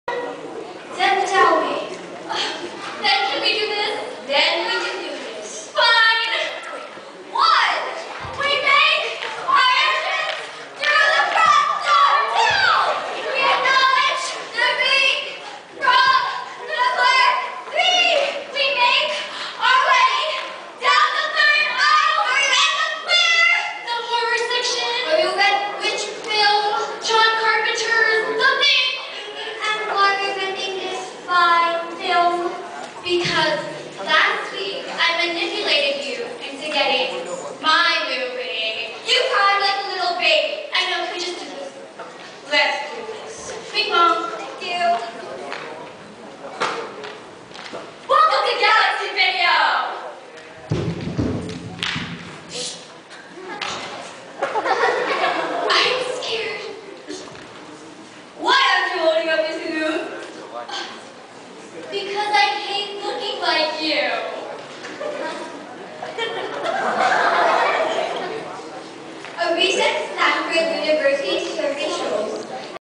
GCA Senior Talent Show: Forgot their names
I forgot their names, but they were awesome.
Thier performance was this interperting scene of some video store they talk about. I was confused at first too. But then I finally got what they were doing. Pretty good talented actresses. They never made their back face us.